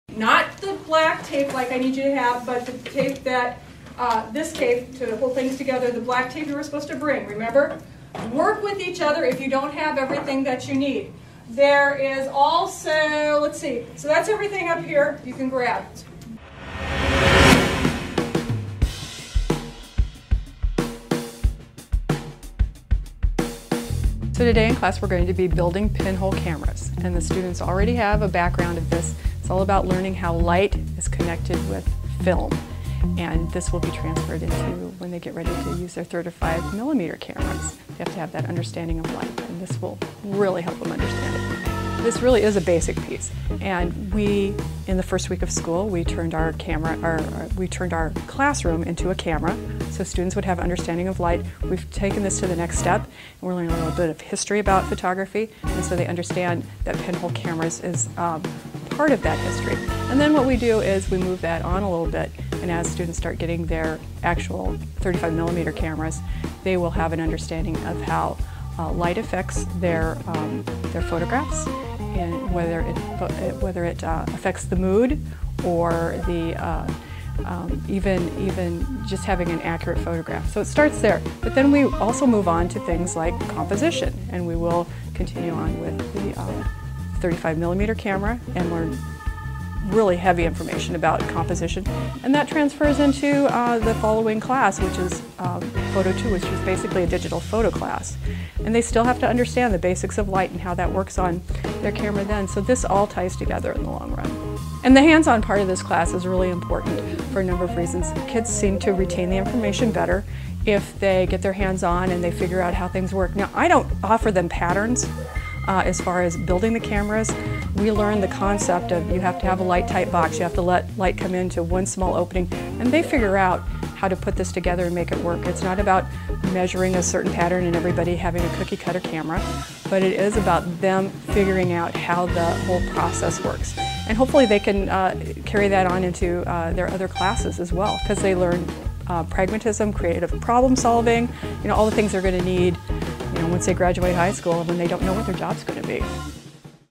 Pinhole Cameras at AHS
Students in Robin Brown's Photography 1 class begin the year building pinhole cameras to learn about the process of photography.